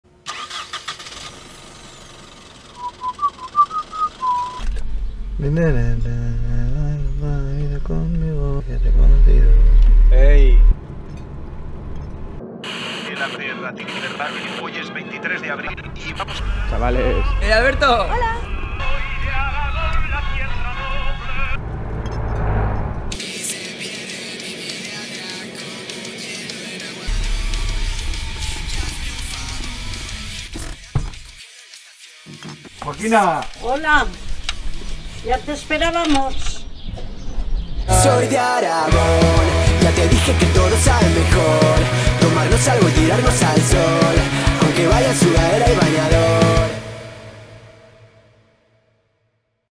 Soy de Aragón | Cervezas Ambar
En Aragón nos chipiamos cuando llueve, cogemos capazos con las vecinas y somos tozudos hasta la médula. Liamos de vez en cuando algún chandrío. A los bobos los llamamos ababoles y a la cerveza, siempre la llamamos Ambar🍻🍻